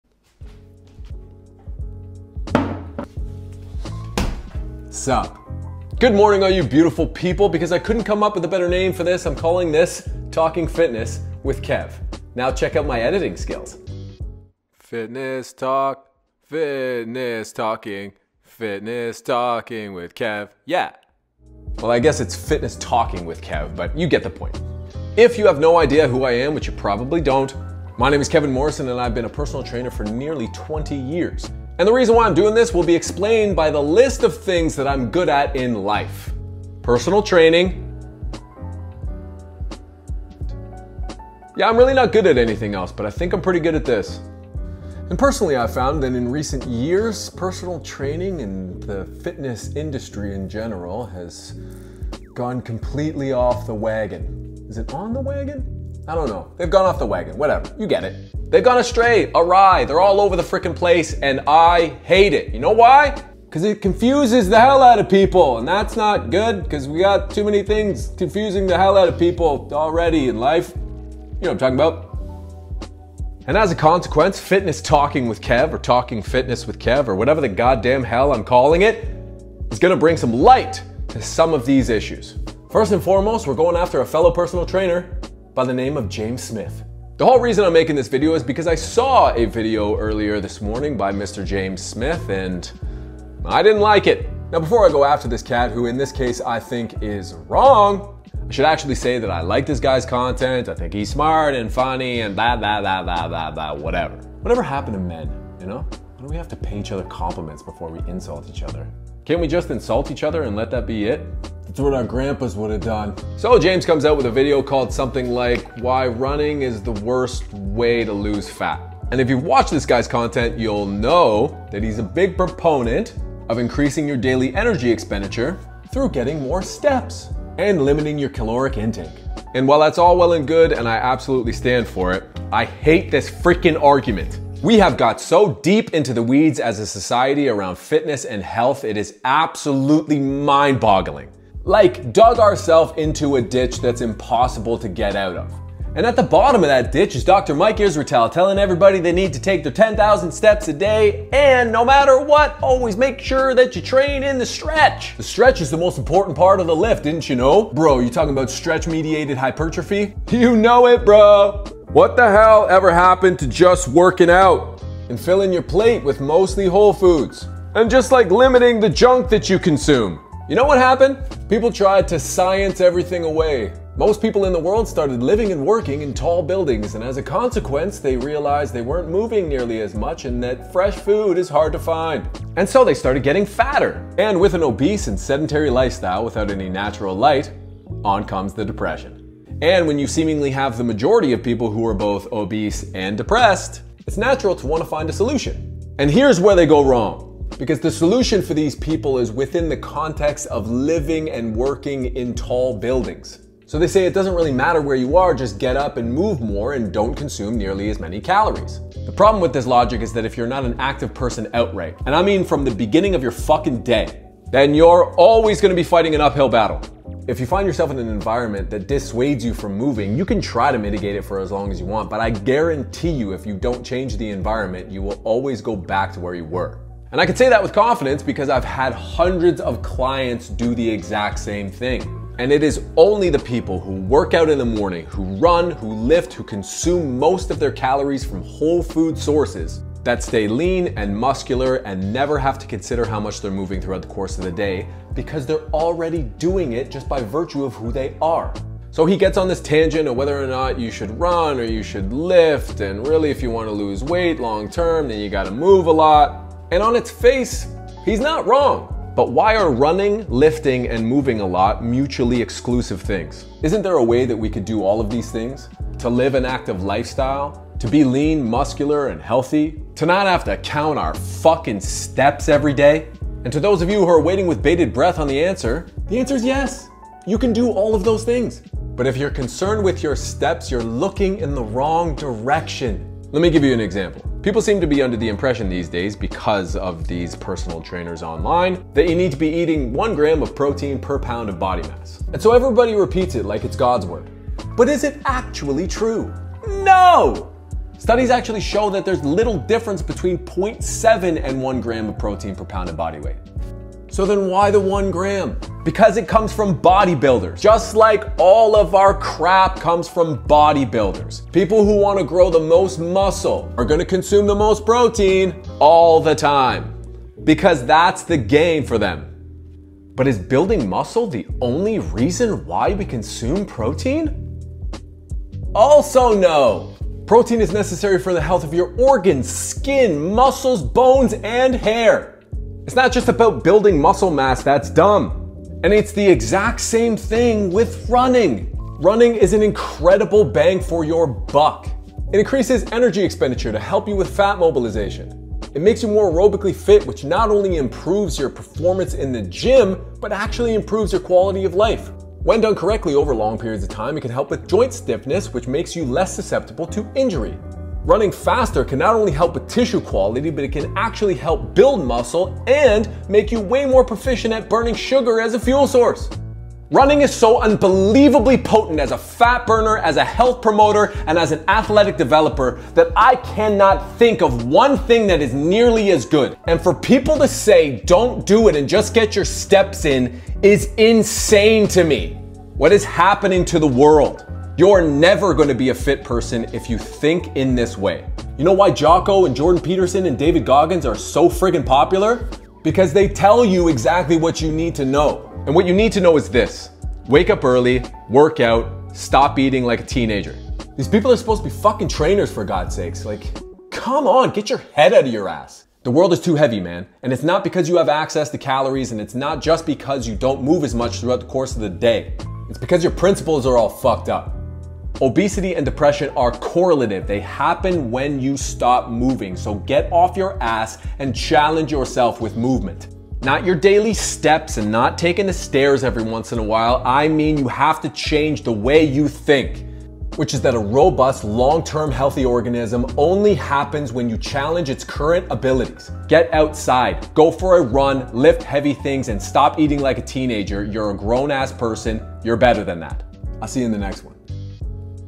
Why Running Is The WORST Way To Lose Weight
There's a lot of confusing information out there about running and weight loss. Let's clear the air.